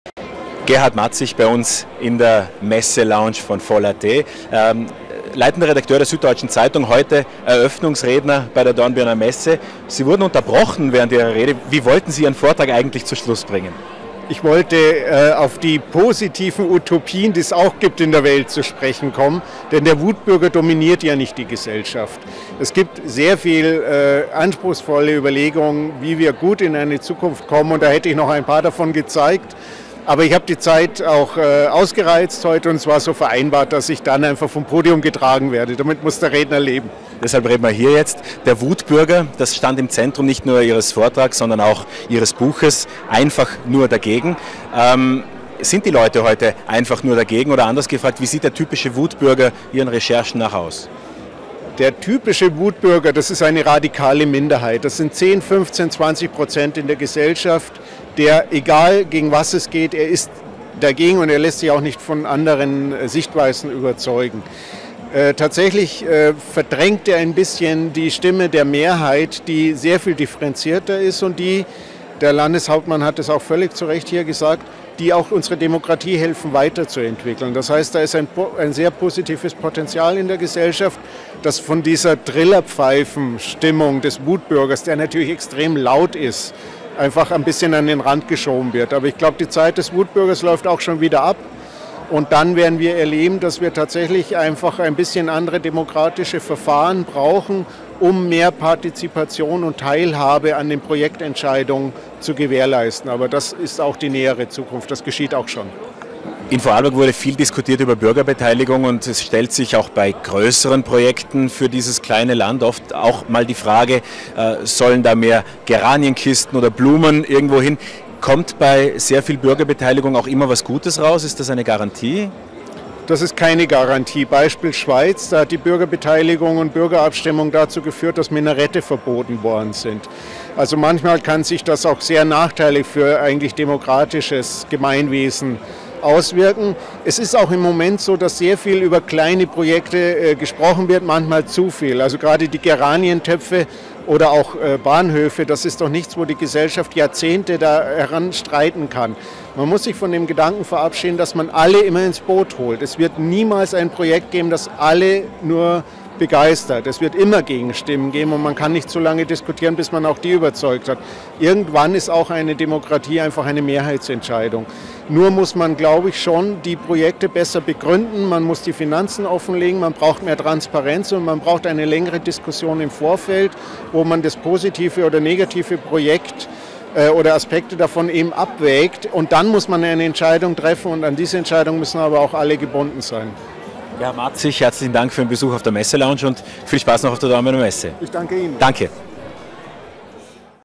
38. Dornbirner Frühjahrsmesse 2013: Gerhard Matzig
Am Donnerstag wurde die Frühjahrsmesse 2013 in Dornbirn eröffnet. Im Interview spricht Russmedia Digital-Geschäftsführer Gerold Riedmann mit Süddeutsche-Redakteur Gerhard Matzig über dessen neuestes Buch - und den Wutbürger an sich.